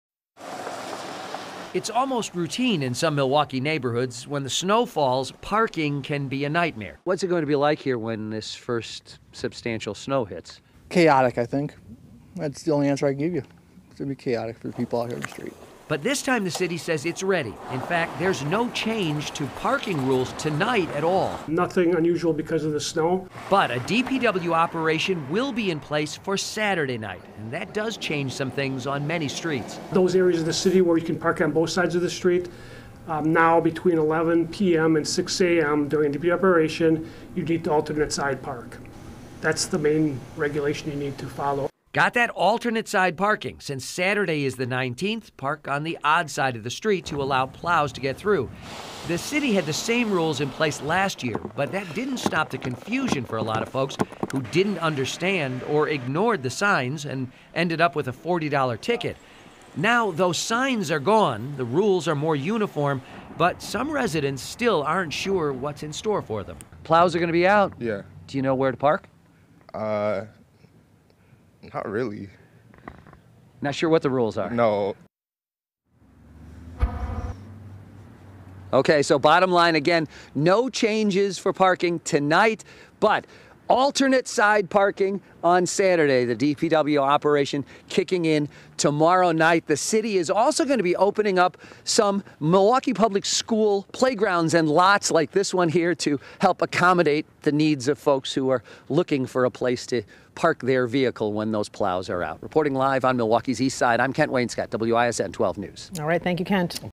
No Plowing Operation Scheduled Friday; Parking Rules Won't Change Until Saturday in Milwaukee
The city says it's ready for the snow, and there will be no change to parking rules Friday night, but, a DPW operation will be in place for Saturday night, and that does change things on many streets.

“Those areas of the city where you can park on both sides of the street, now between 11 p.m. and 6 a.m. during a DPW operation, you need to alternate side park. That's the main regulation you have to follow,” Milwaukee parking director Richard Dollhopf said.

Since Saturday is the 19th, people should park on the odd side of the street to allow plows to get through.